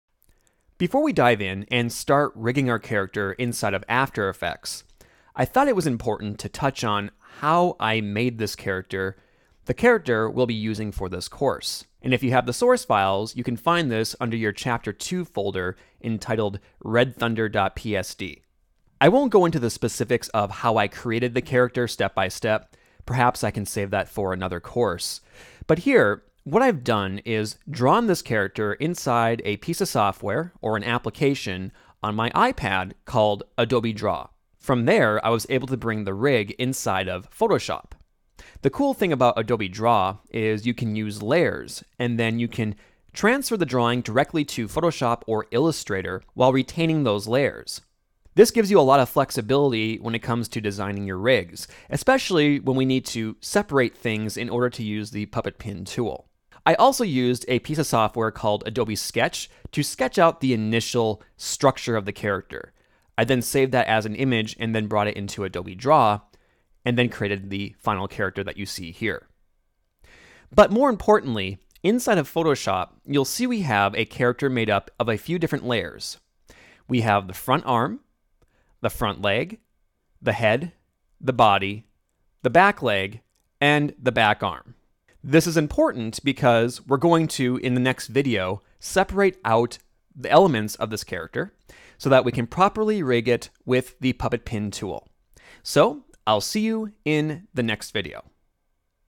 Overview of Rig (2/19) - After Effects CC: Puppet Pin Walk Cycles
This is part 2 of 19 of the After Effects CC: Puppet Pin Walk Cycles course.

In this video I discuss how I created the character we will use to create a walk cycle. Characters can be designed in a number of ways!